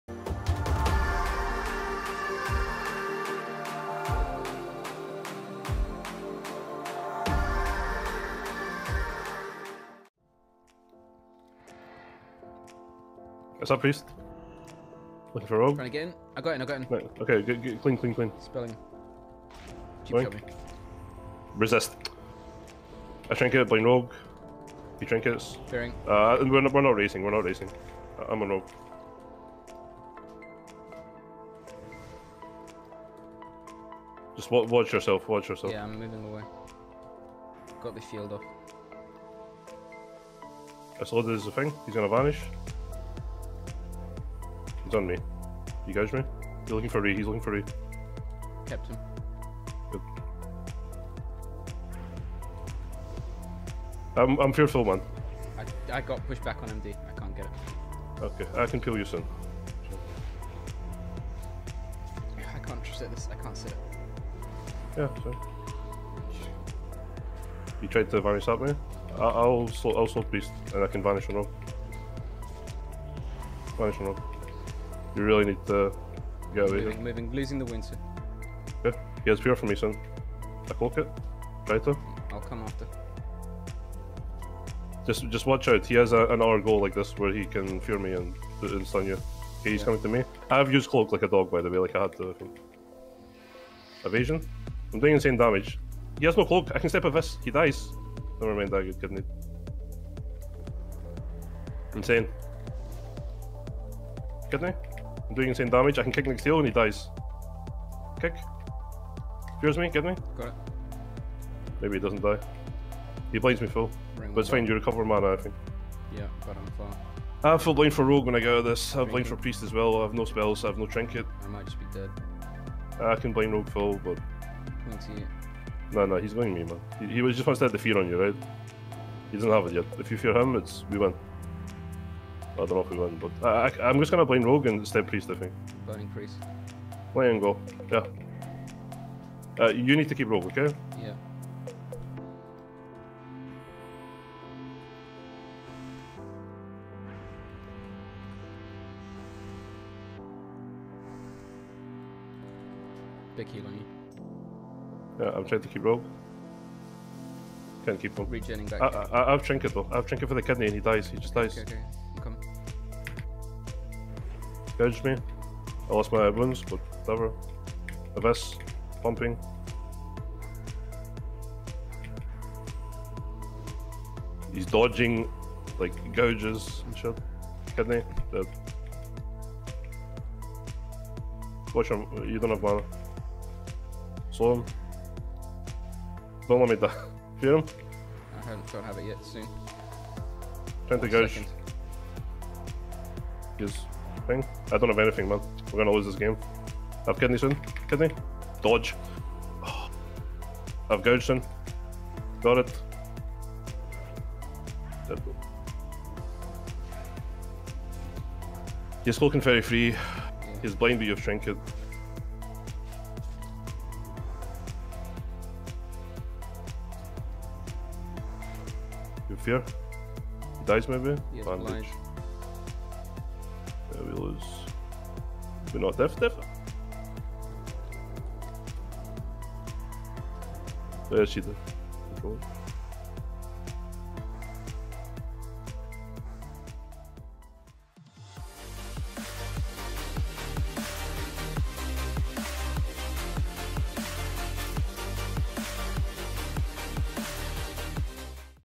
The Closest Mirror of Season 3
Insanely close Priest Rogue mirror against another rank 1 Priest Rogue.